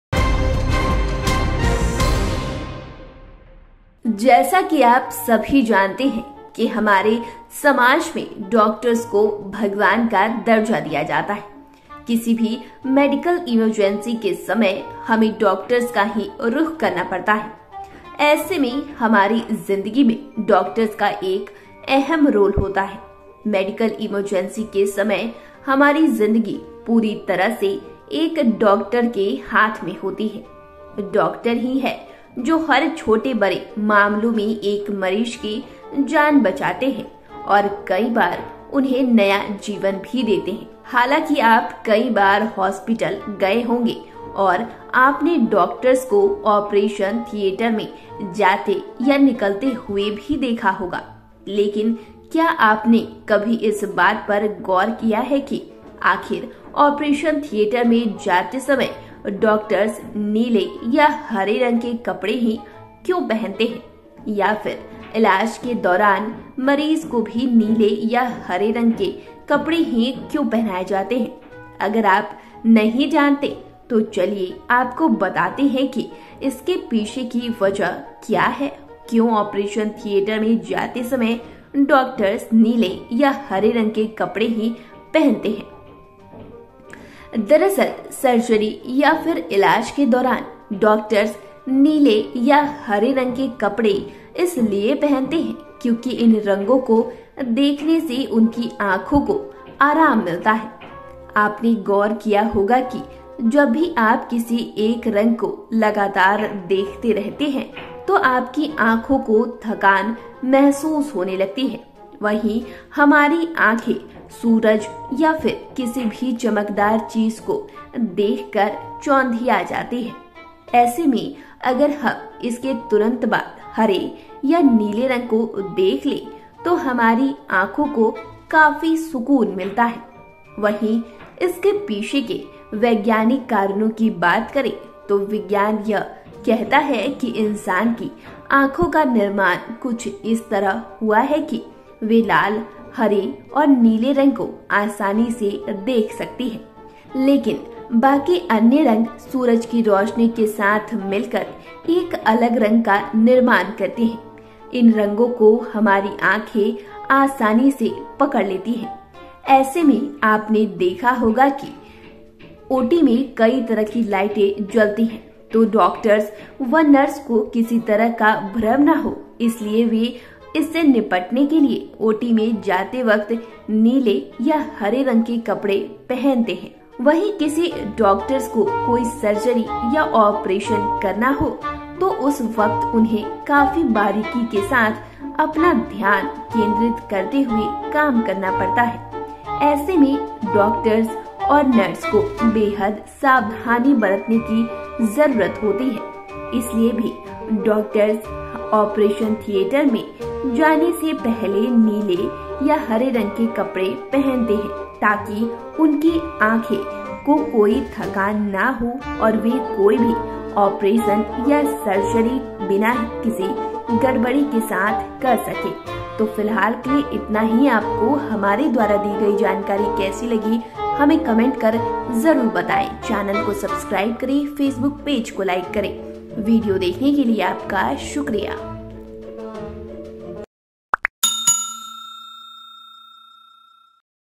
Doctors Operation theatre में हरे या नीले रंग के कपड़े ही क्यों पहनते हैं? वजह हैरान करने वाली
डॉक्टर्स ऑपरेशन थियेटर में हरे या नीले रंग के कपड़े ही क्यों पहनते हैं..? वजह हैरान करने वाली..देखें वीडियो...